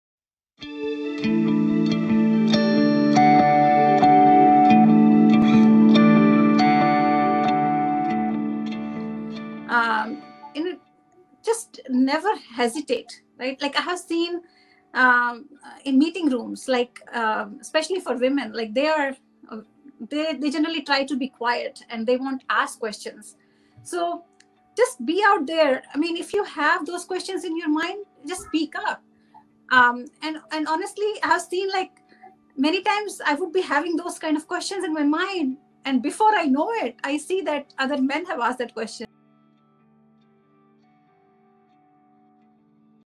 Gaining visibility in the workplace
Got something to say in a team meeting? Don't let fear hold you back. Expressing your viewpoints and asking relevant questions can accelerate your visibility in the workplace.

In this ThroughlineThursday conversation with host Neha Malhotra, our guest Sunita Sharma, Director of Software Development, Oracle tells us why it's important for women to speak up.